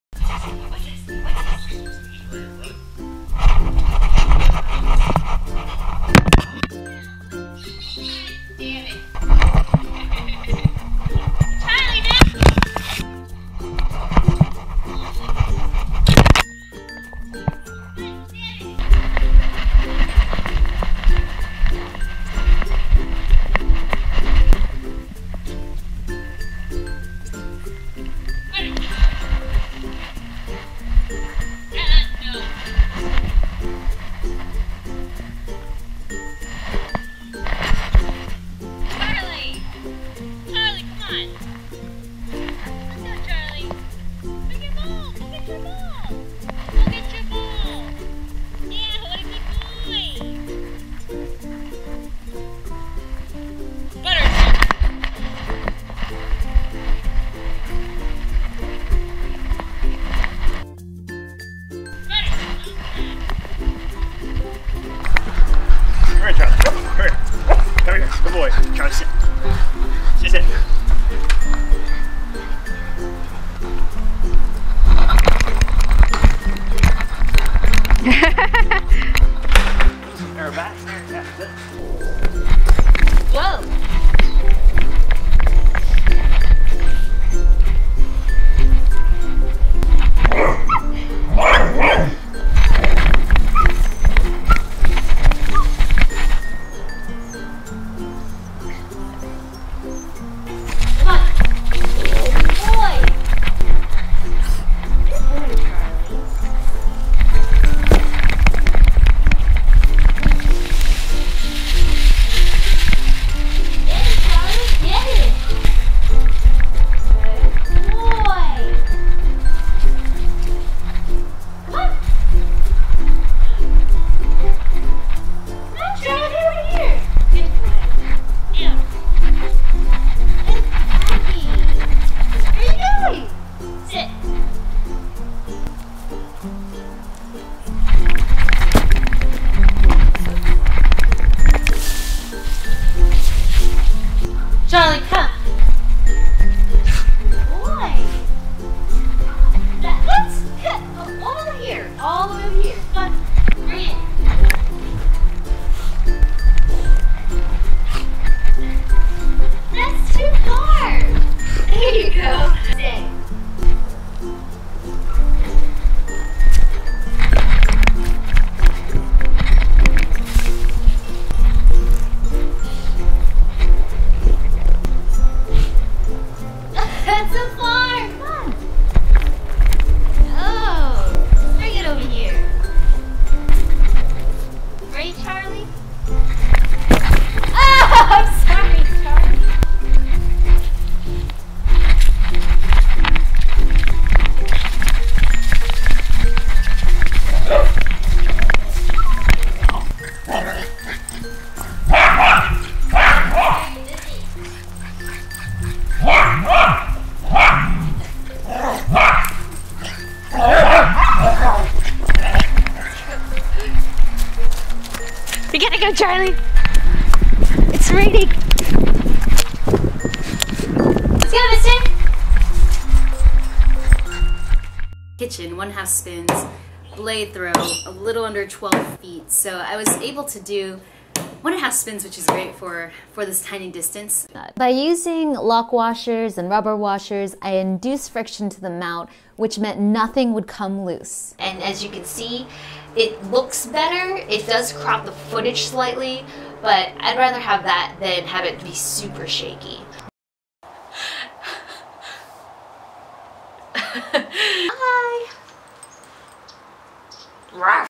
My DIY Dog GoPro Mount in Action (+ Kick Fetch Ball)
This video shows my DIY Dog GoPro Jacket mount in action (particularly after 1:22 with the addition of the rubber sleeve).

If you’d like to know how I made this mount, check out the video below:

In this video, I talk about how to reduce the shakiness of your dog footage